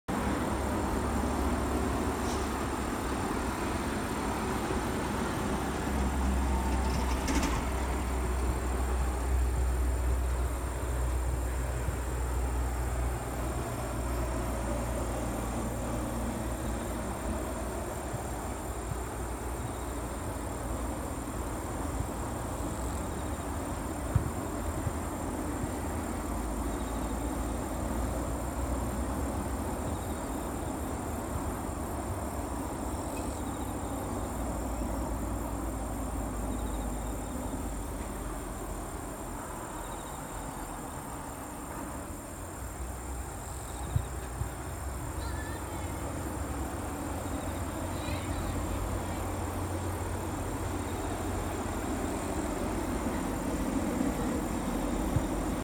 のどかな 川風景。鴨家族
鴨？の家族が、気持ち良さそうに。